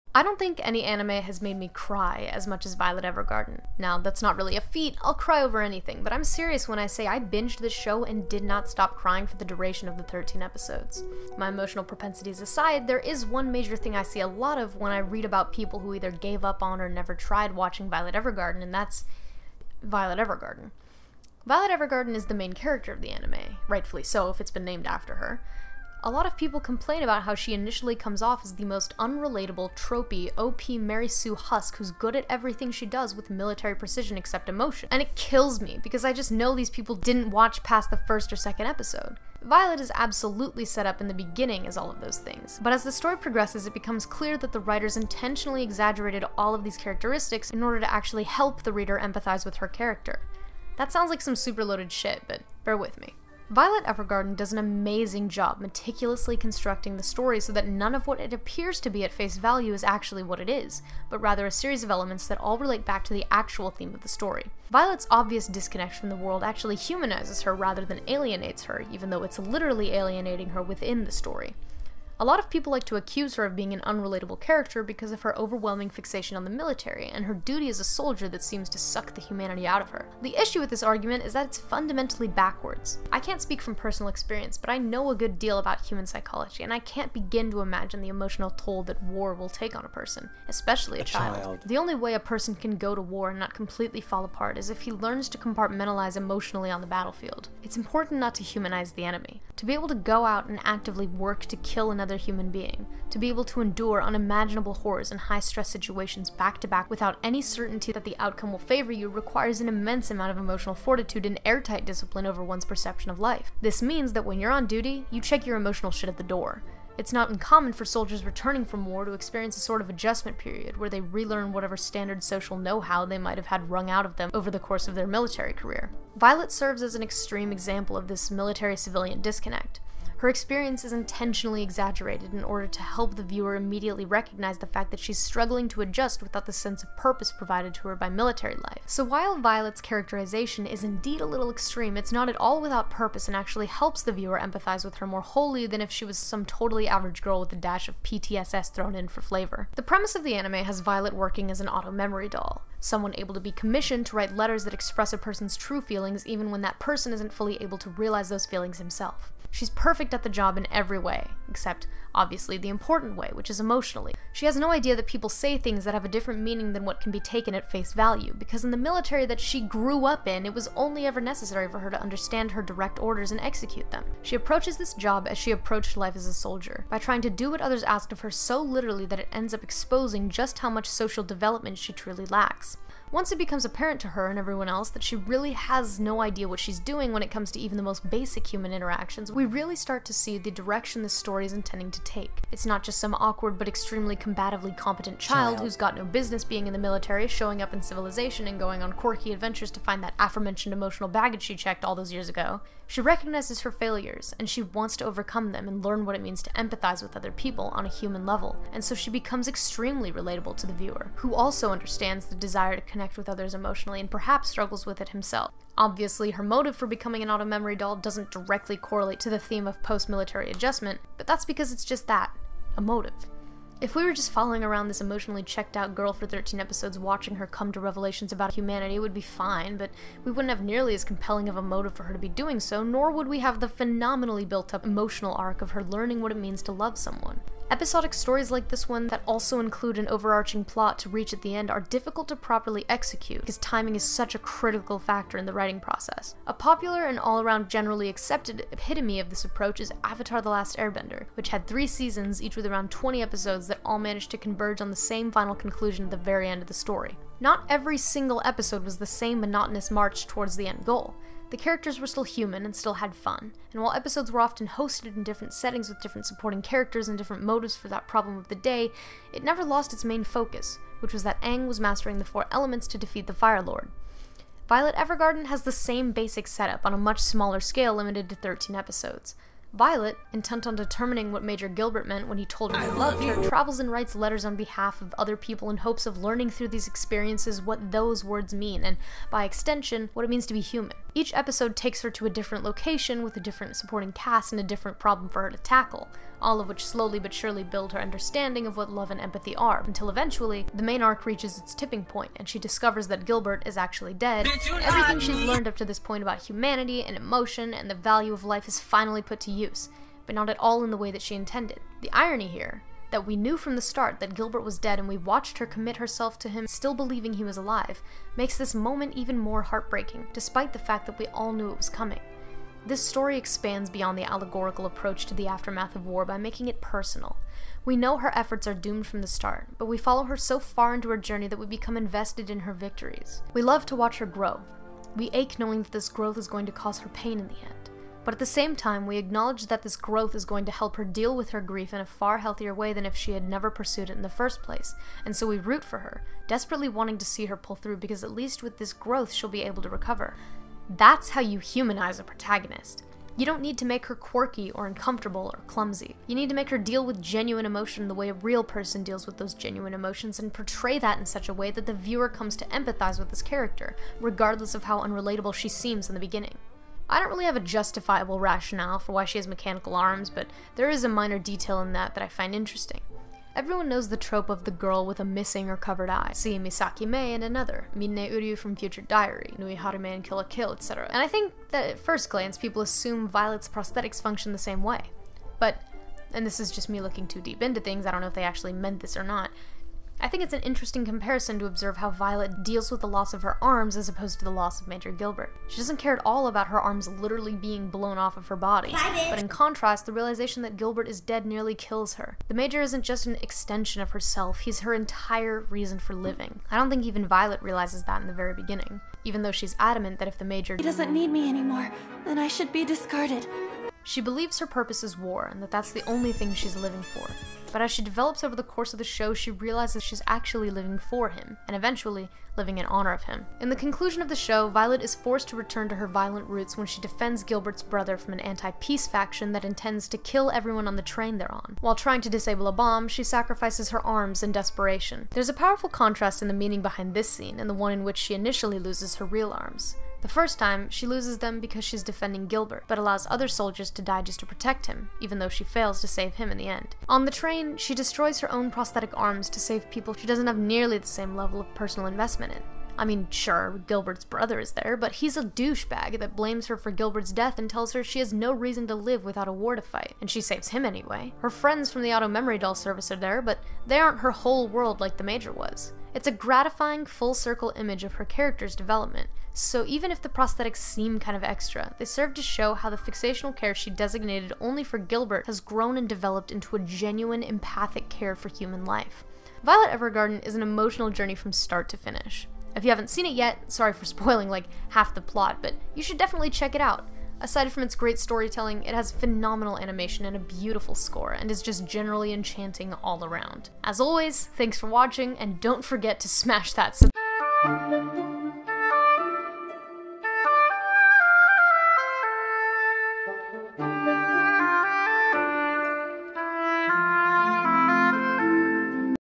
Violet Evergarden is a Relatable Character
today avo claps back at one of the biggest reasons people give up on violet evergarden